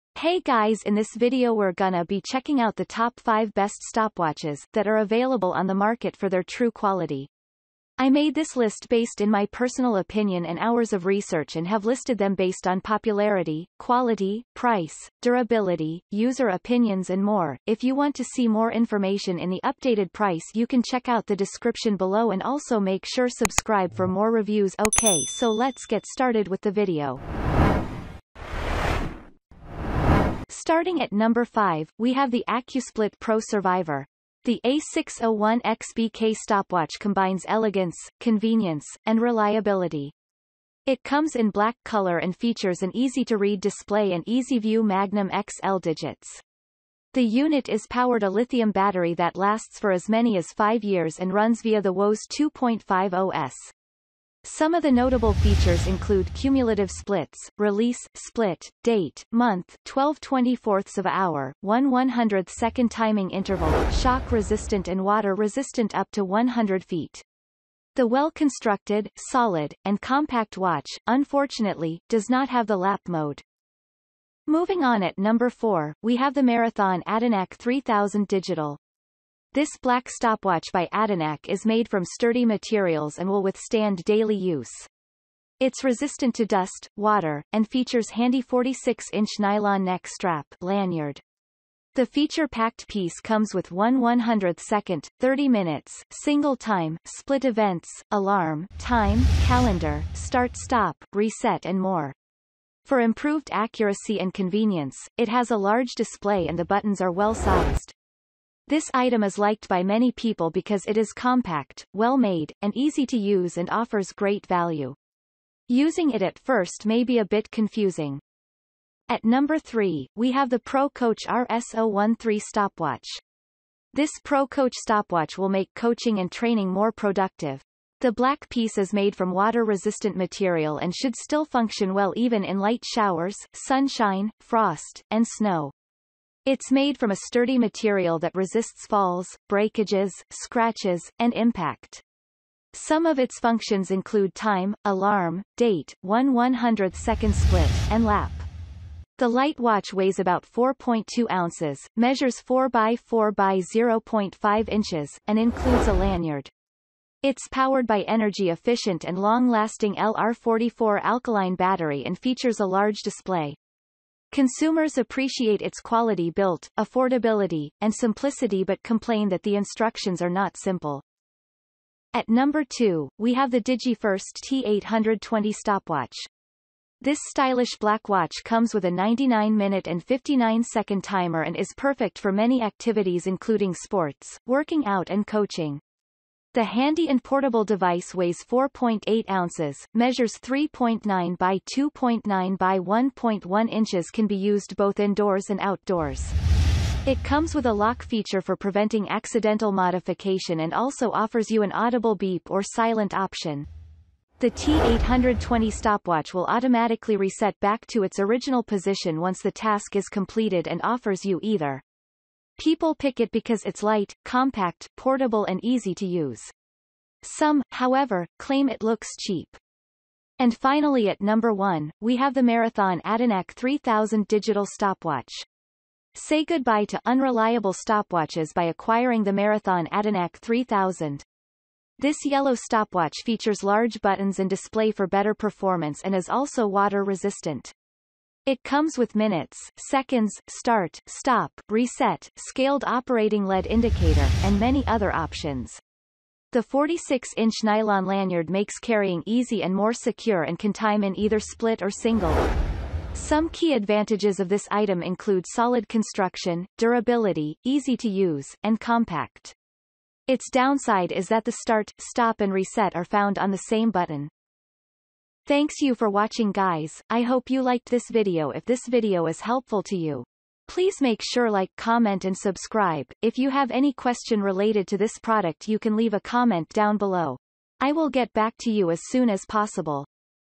Top 5 Best Stopwatches in 2024
▶️ 5.  ACCUSPLIT Pro Survivor

▶️ 4.  Marathon Adanac 3000 Digital

▶️ 3. ProCoach RS-013 Stopwatch

▶️ 2.  Digi 1st T-820 stopwatch

▶️ 1.  Marathon ADANAC 3000 Digital Stopwatch